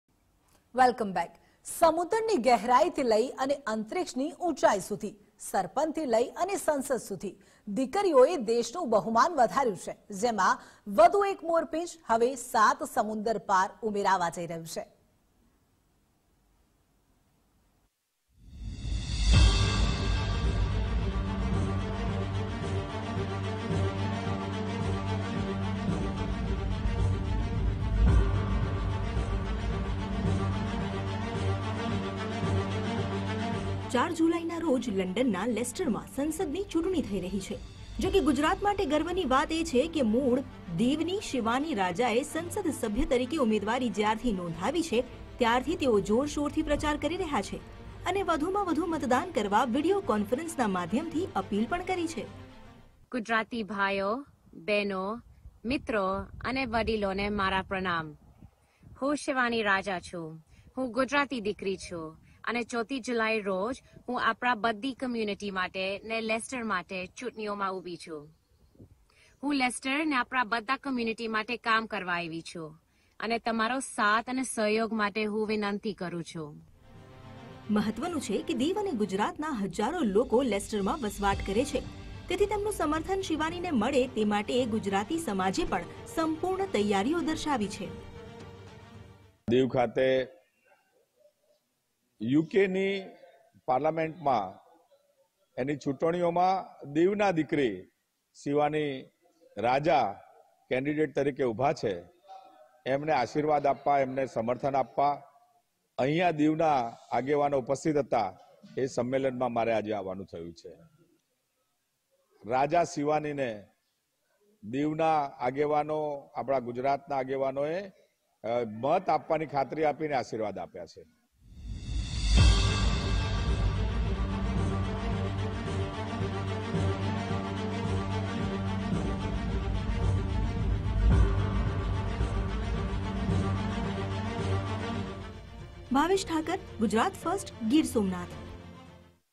U.Kમાં Election ઉમેદવાર Shivani Raja એ સંસદ સભ્ય તરીકે ઉમેદવારી કરી | Gujarat First
- શિવાની રાજાએ સંસદ સભ્ય તરીકે ઉમેદવારી કરી
- વીડિયો કોન્ફરન્સના માધ્યમથી મતદાનની કરી અપીલ
- ગુજરાતી સમાજે સમર્થનનું આપ્યું આશ્વાસન